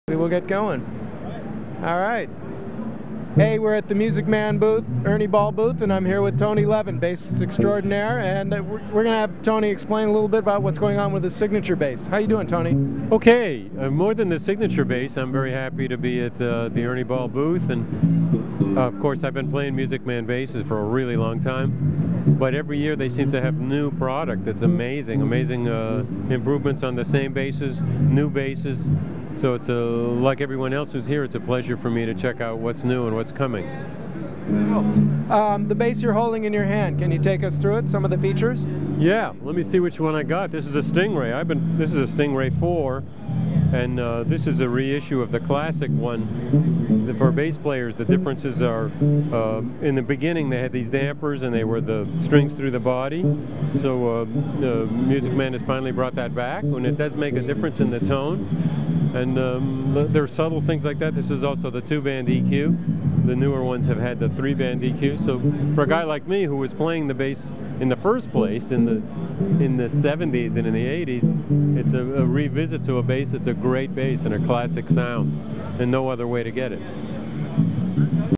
Tony Levin at the MusicMan booth at Winter NAMM 2010
Interview with Tony Levin